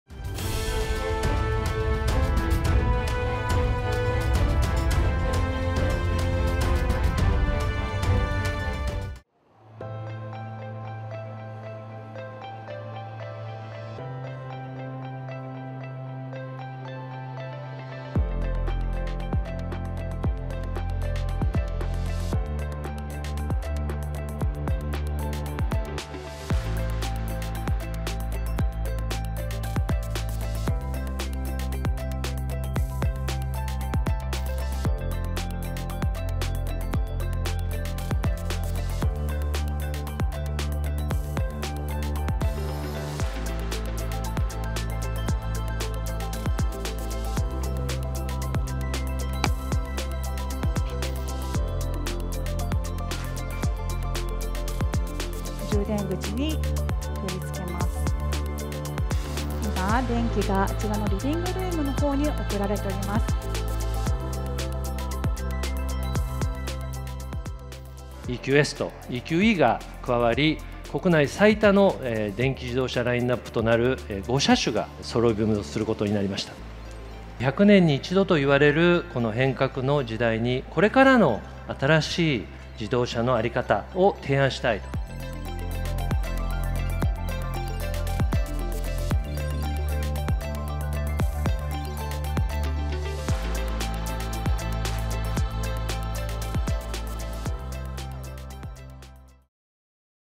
メルセデス、初の高級EV「EQS」販売開始 航続距離は700キロ
メルセデス・ベンツ日本が電気自動車（EV）の「EQS」「EQE」の２車種を発表しました。EQブランドとして日本で発売しているEVはいずれも多目的スポーツ車(SUV）で、SUV以外のモデルは初めてです。「EQS」の特長は航続距離700キロメートルで、国内で販売されているEVでは最長といいます。

※日本経済新聞社の動画について改ざんや、許可なく商用・営利 目的で利用することを禁じます。EQS　ベンツ